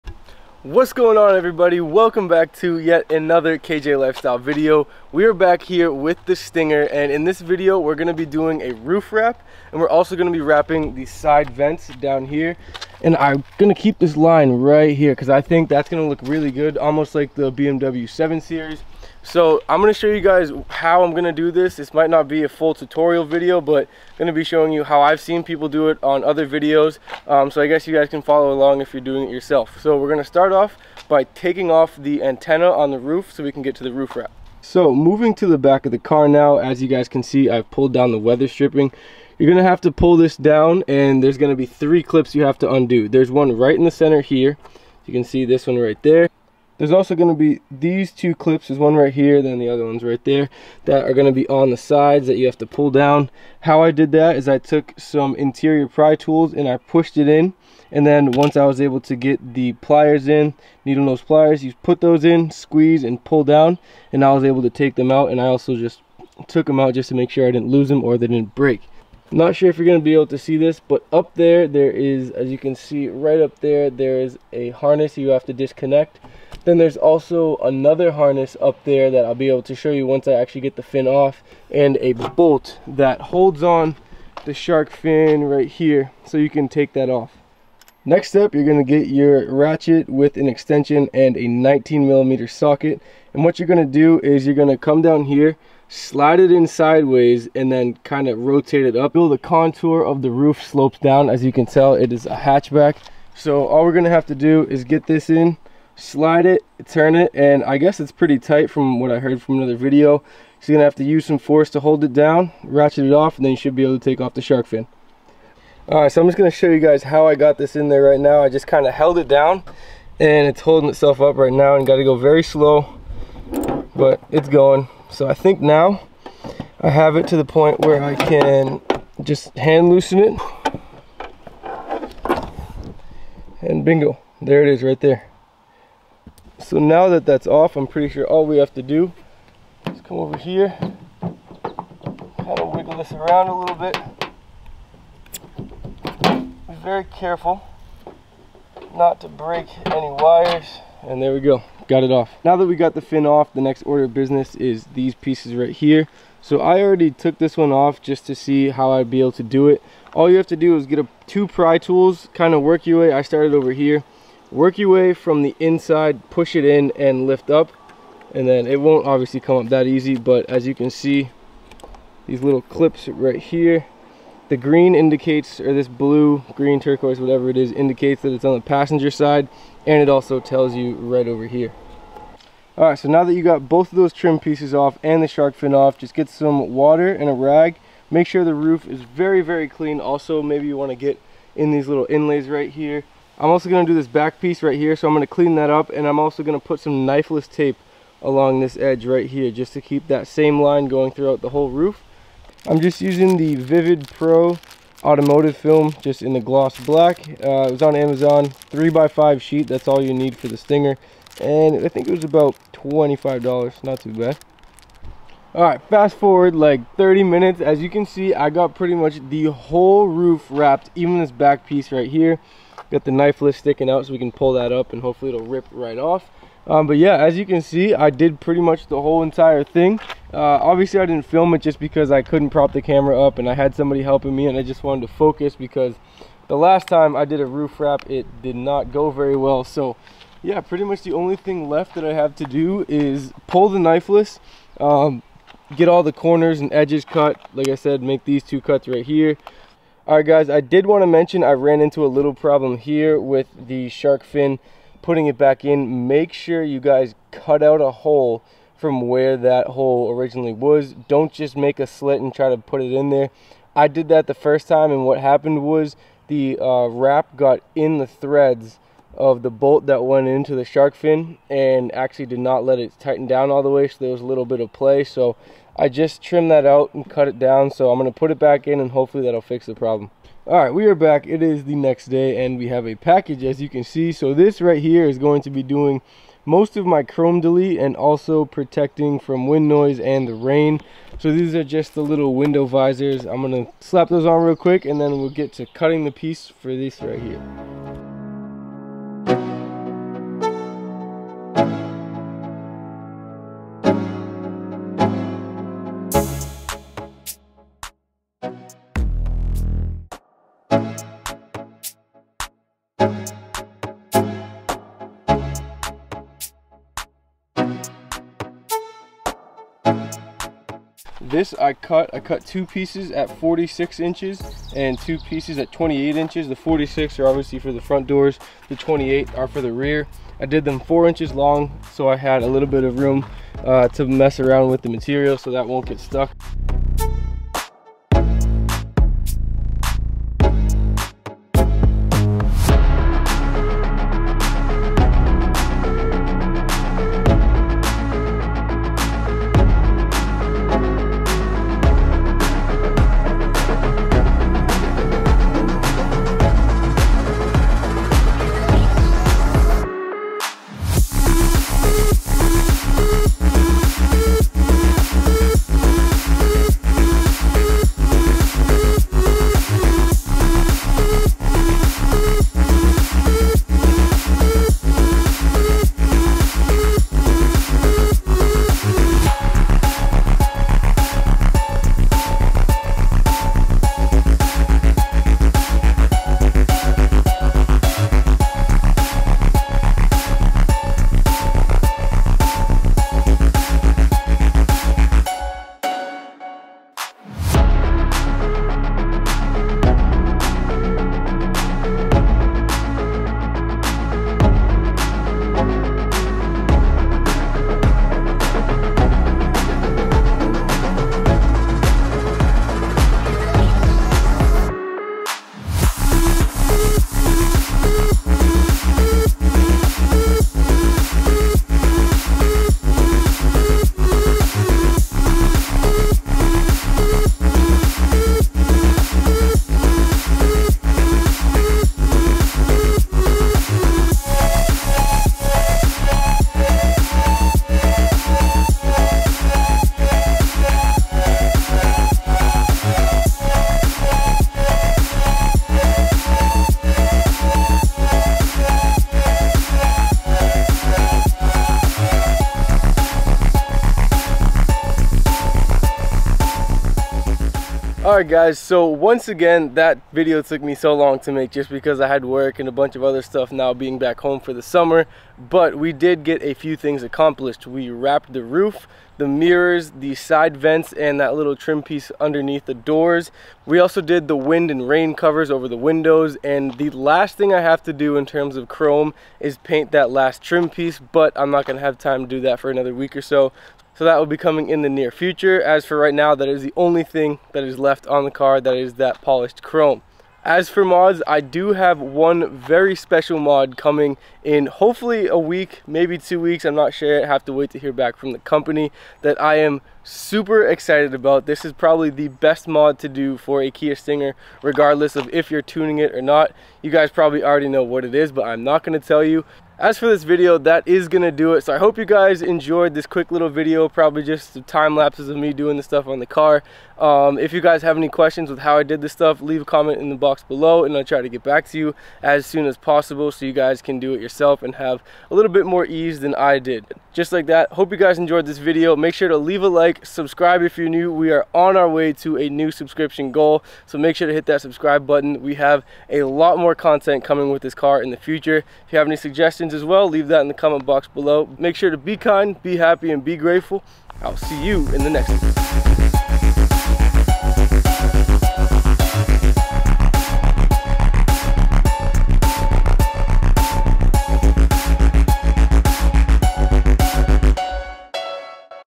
BEST Kia Stinger Exterior Mods Under $100-Roof Wrap Tutorial, DO THIS!
These are the best exterior mods for the Kia Stinger that you can do yourself for under $100! In my opinion it makes the car look a lot more aggressive and a bit more personality. The roof wrap, side vents and trim were fairly easy but the mirrors were a bit more difficult! I am not specialized in wrapping but with some time and patience was able to get it done. Let me know if you have any questions and all tools/materials can be found here:

Auto Clover Wind/Rain Guards:

Vinyl Card:

Heat Gun:

Interior Pry Tools:

Be Kind, Be Happy, Be Grateful!
“Live Your Lifestyle”